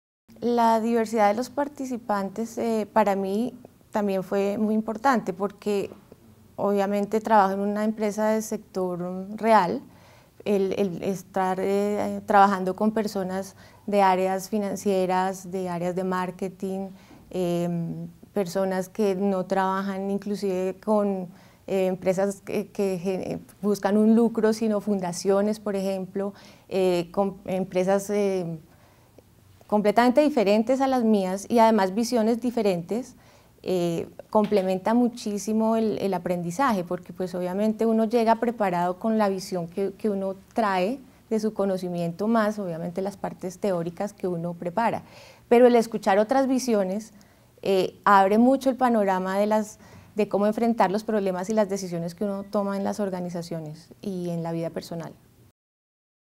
Diversidad - Dra. Diana Ramírez - MBA INALDE Business School
La Doctora Diana Yamile Ramírez, Gerente Financiero Regional de Argos Colombia, y egresado del Programa de Maestría en Administración de Negocios (MBA) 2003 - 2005, habla de su experiencia sobre la diversidad de aprendizaje, en INALDE Business School.